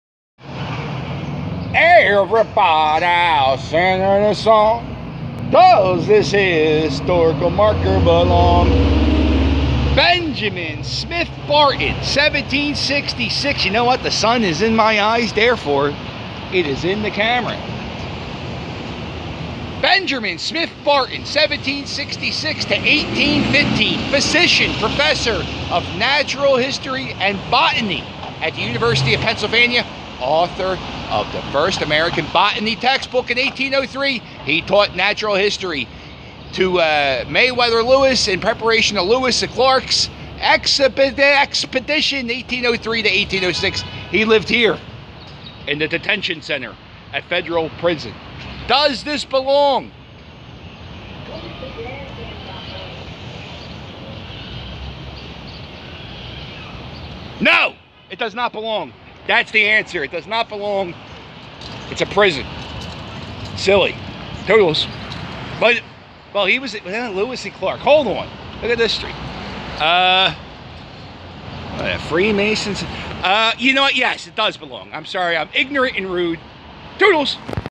Does This Historical Marker Belong?
I see a sign, I read a sign and I decide if we need that sign.